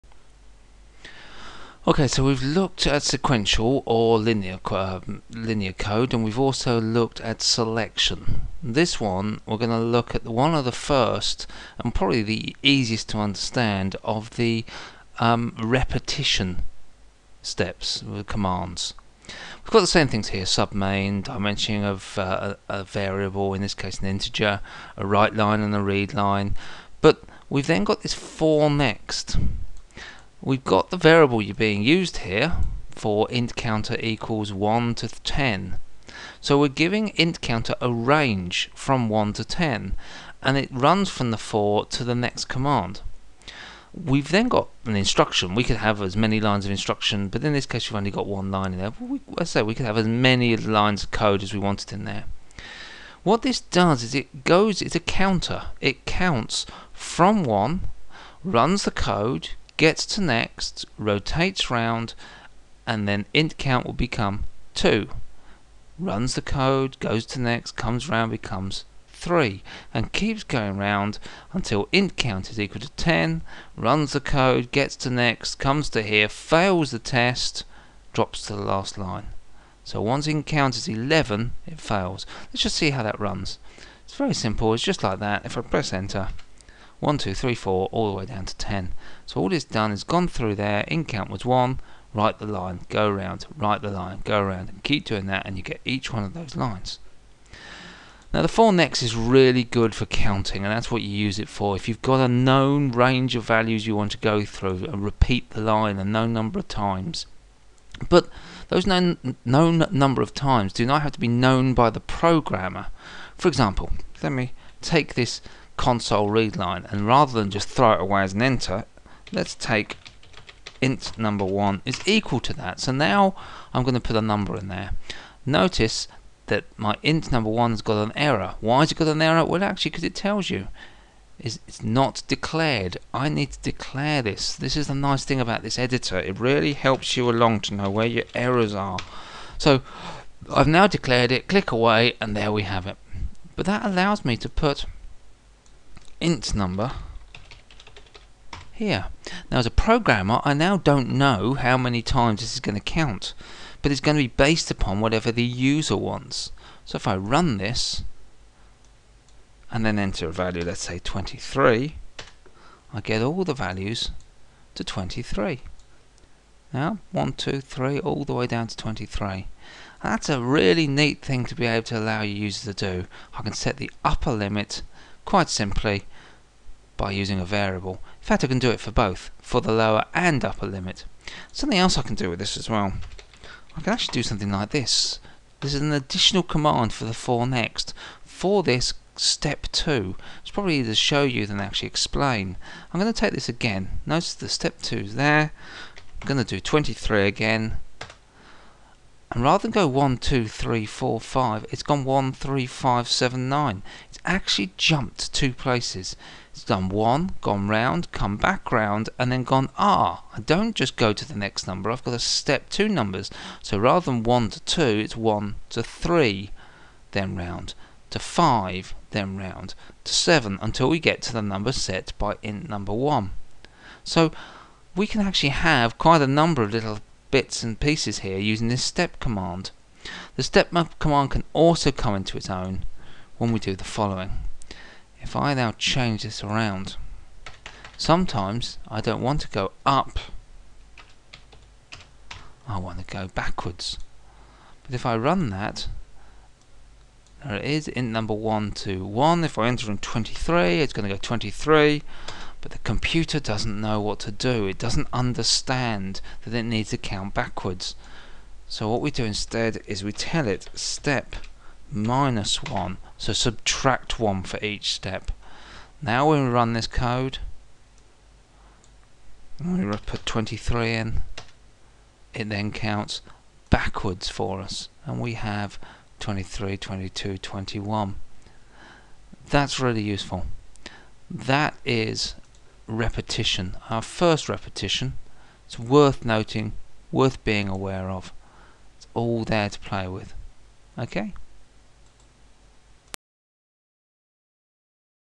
VB Programming 5 - For...Next
How to utilise a For...Next Loop in Visual Basic